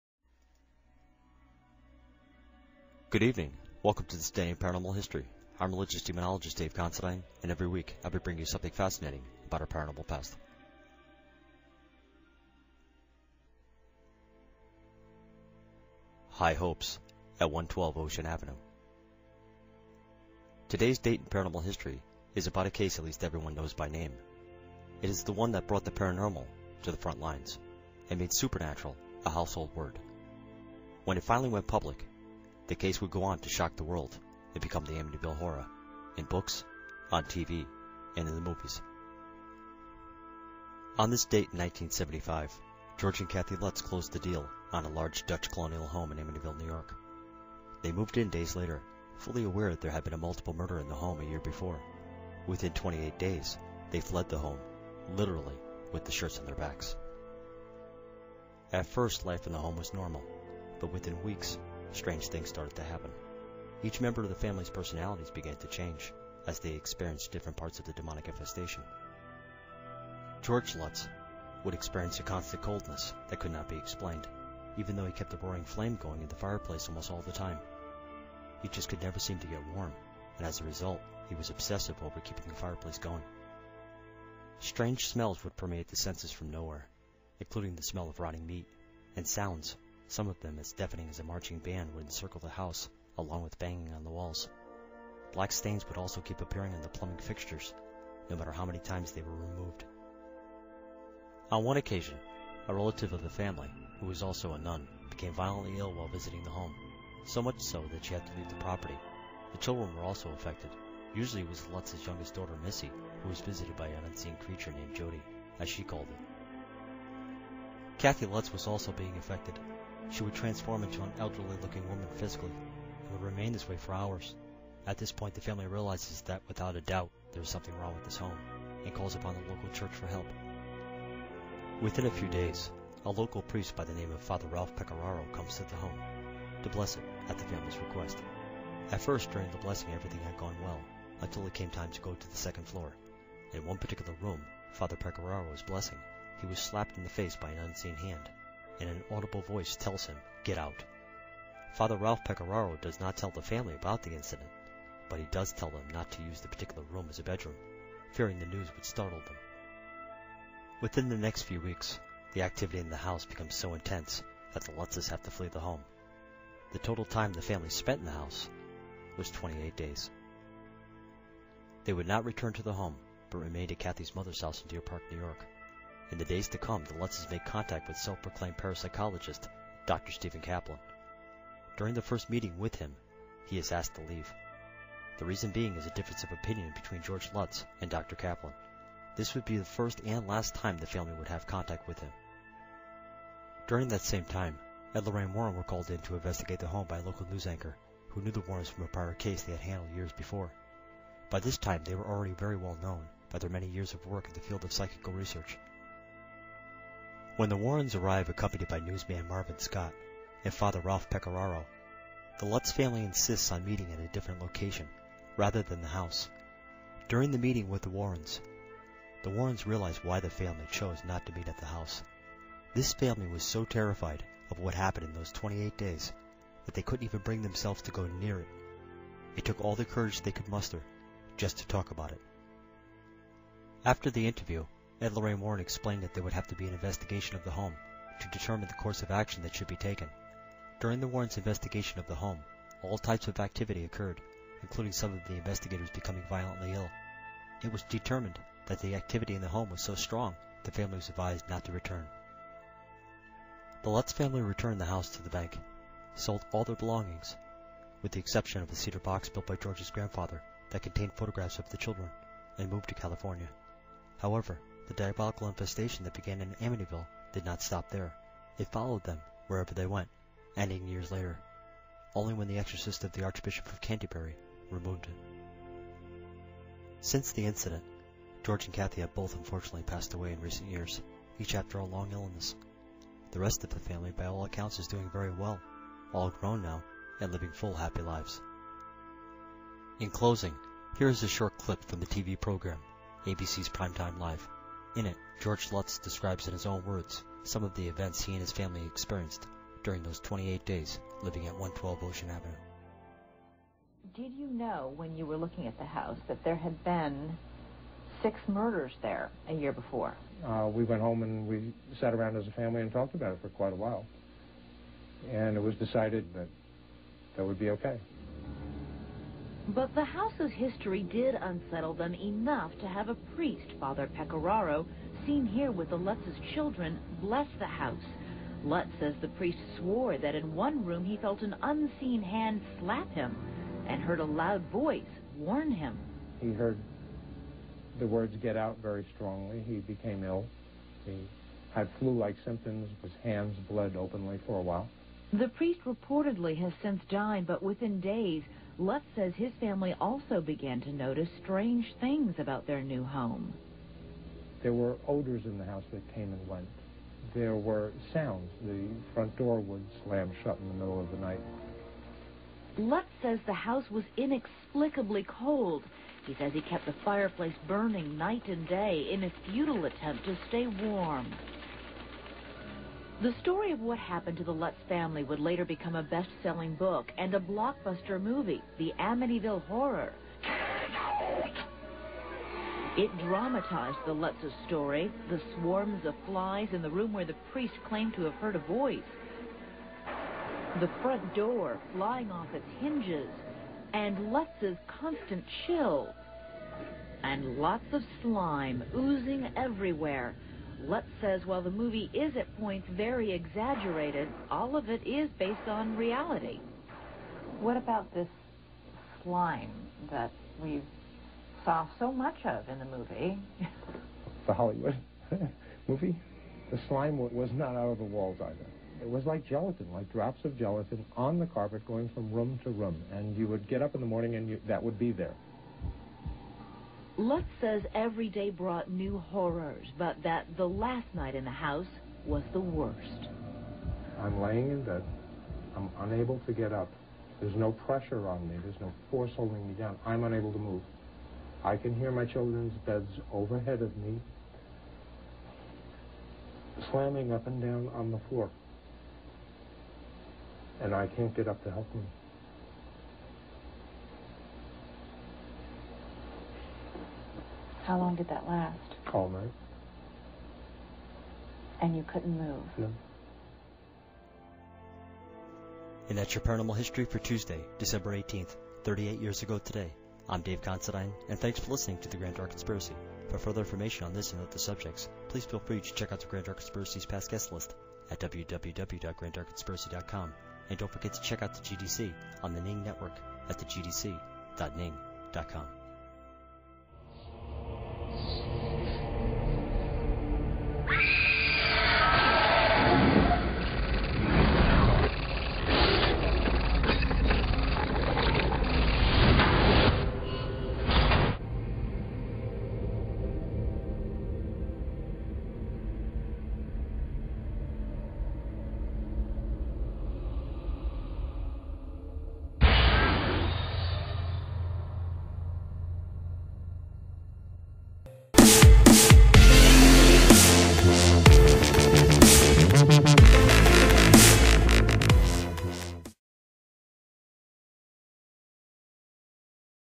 This Day In Paranormal History 12 -18 -2018 (The Amityville Horror, High Hopes at 112 Ocean Avenue)
On December 18, 1975, George and Kathy Lutz closed the deal on a large Dutch colonial home in Amityville, New York. They moved in days later, fully aware that there had been a multiple murder in the home a year before. Within 28 days, they fled the home, literally with just the shirts on their backs...

"The diabolical forces are formidable. These forces are eternal and they exist today. The fairy tale is true. The devil exists, God exists. And for us, as people our very destiny hinges upon which one we elect to follow." - Ed Warren

Rest In Peace George & Kathy, may God bless you both and your children Danny, Christopher and Missy forever and always+